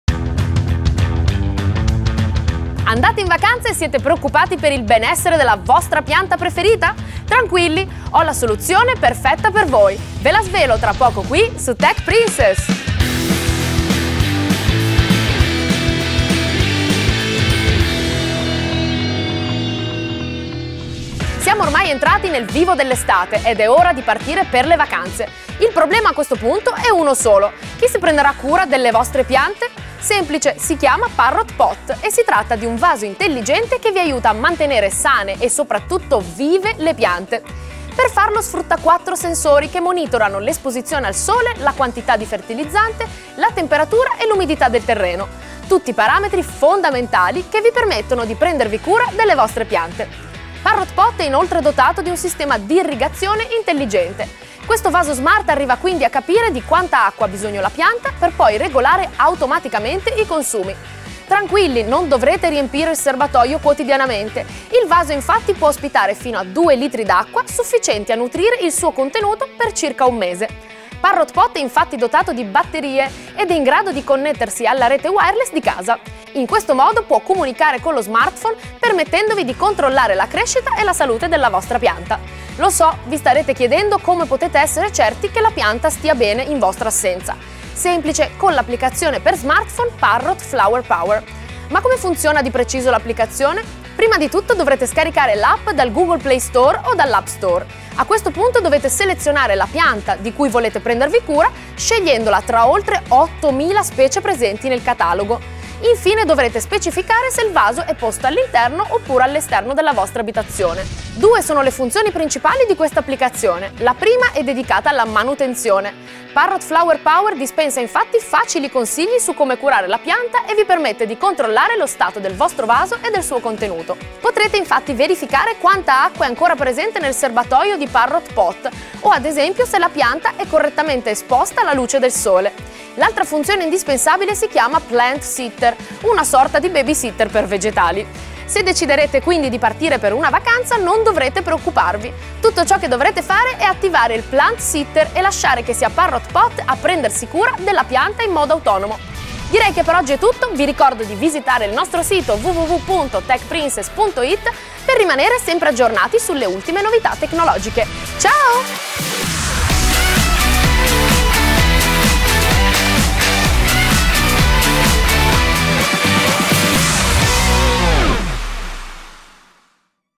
Parrot Pot: che cos'è e come funziona il vaso smart? (01x031) 📺
Tech Princess sbarca finalmente in TV! 📺
In questa puntata la nostra Fjona Cakalli vi parlerà di Parrot Pot, il vaso intelligente in grado di prendersi cura delle vostra pianta preferita senza farla morire. Ecco come come funziona!

💰 ACQUISTA QUI